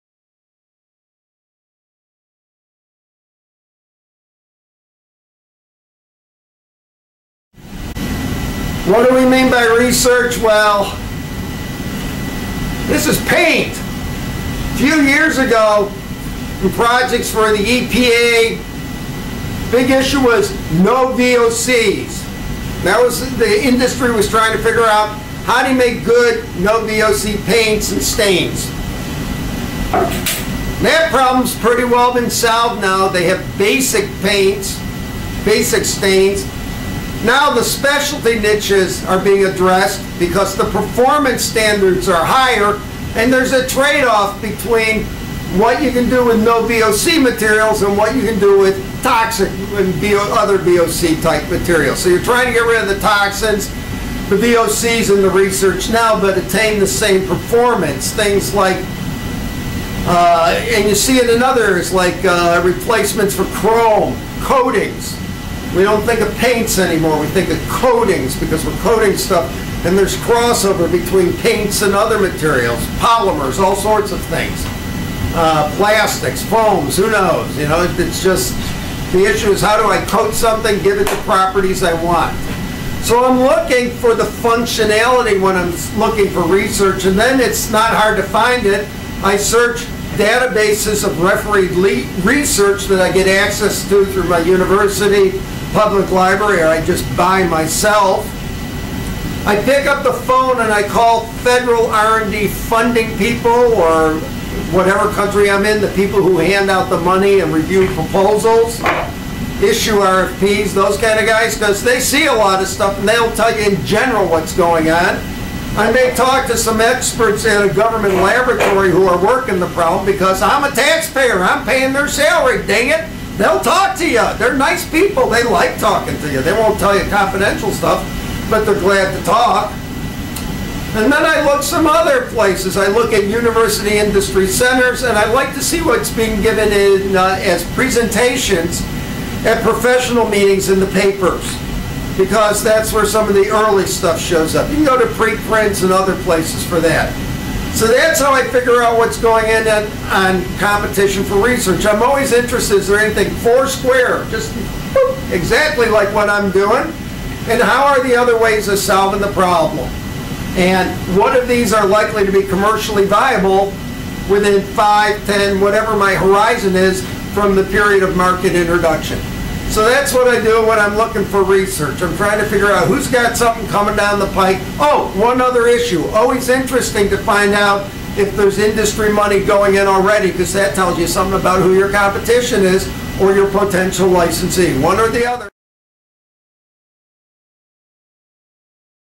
T2+2™: Competition Research
This is Part 2.d.iv video lesson of T2+2™, the new Tech Transfer software from Foresight Science & Technology™